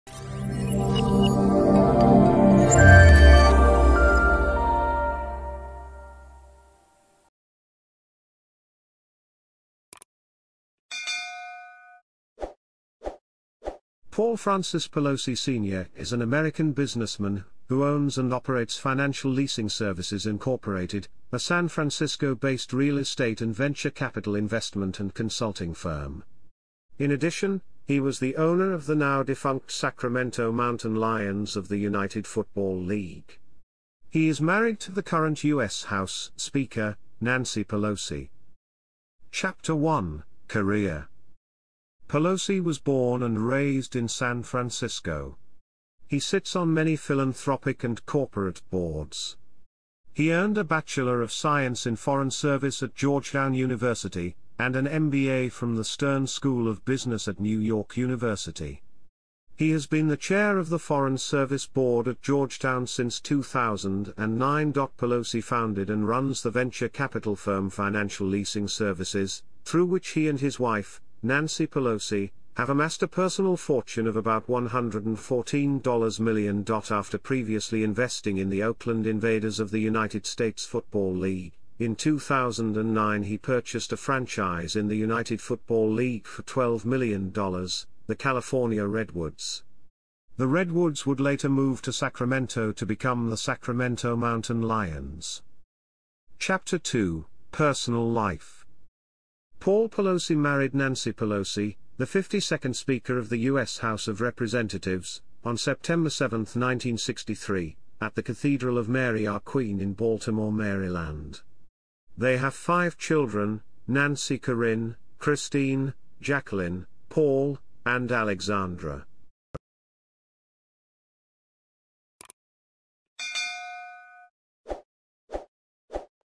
Paul Pelosi - Wikipedia article
Video summary:
Paul Francis Pelosi Sr. (born April 15, 1940) is an American businessman who owns and operates Financial Leasing Services, Inc., a San Francisco-based real estate and venture capital investment and consulting firm. In addition, he was the owner of the now-defunct Sacramento Mountain Lions of the United Football League. He is married to the current U.S. House speaker, Nancy Pelosi.